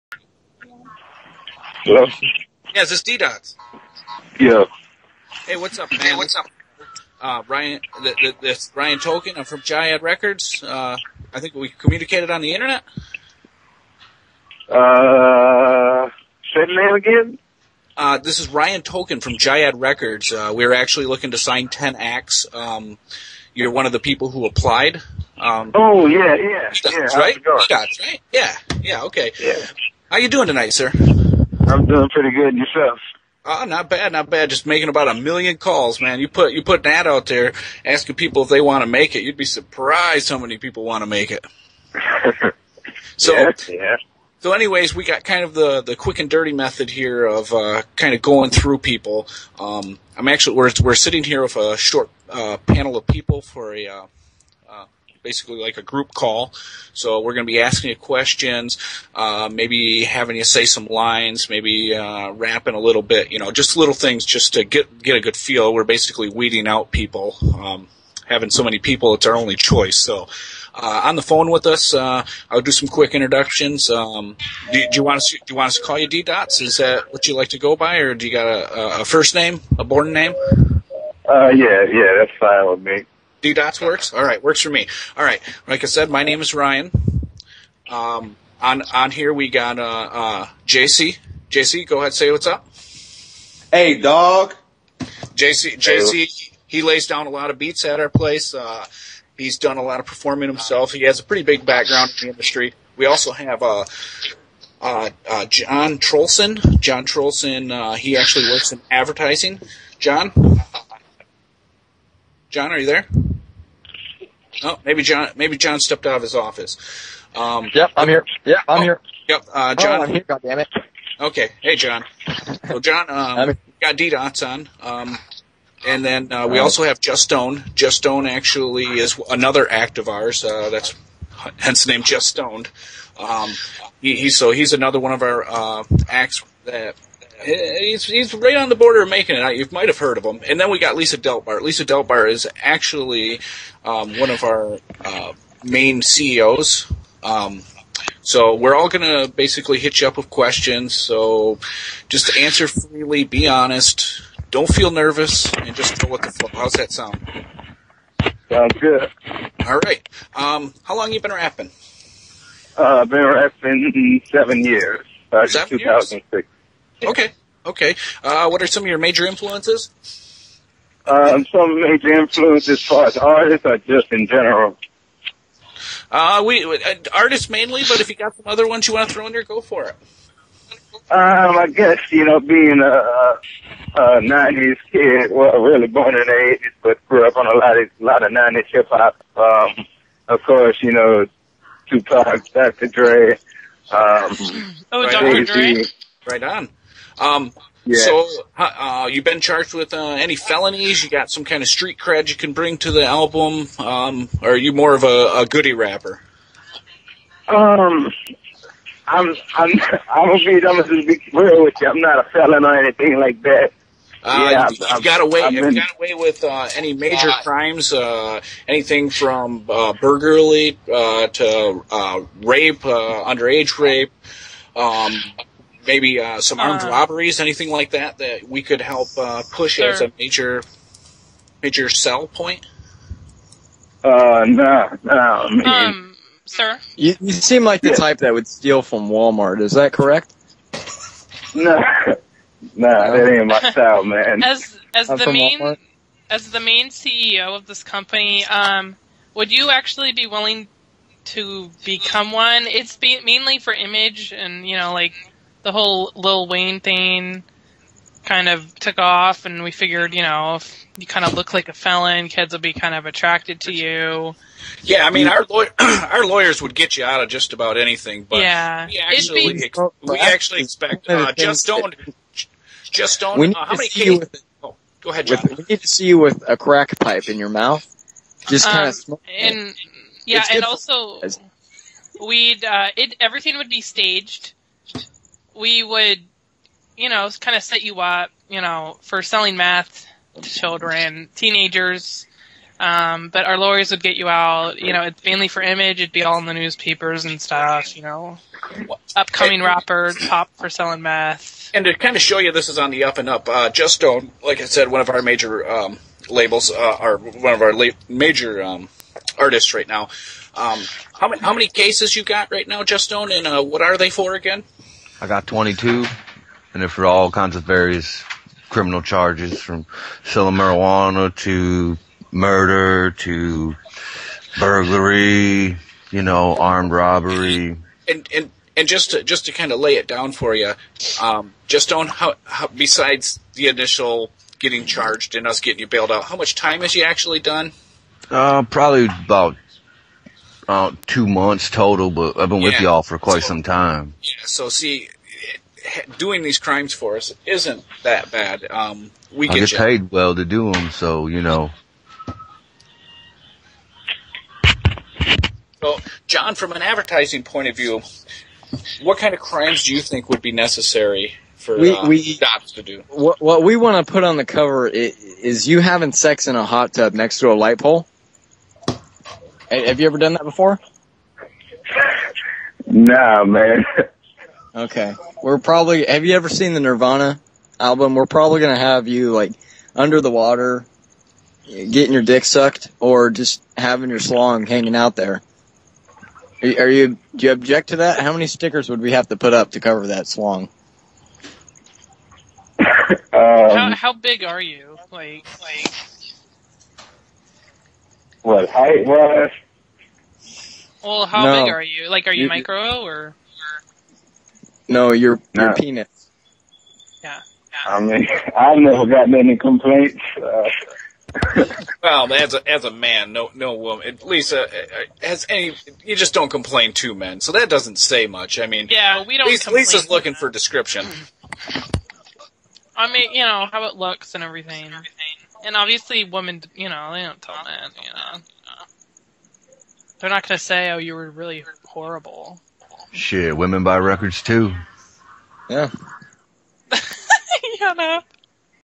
Rapper Audition: Studio Gansta
Another rapper call, but this time we try to make a guy sell out.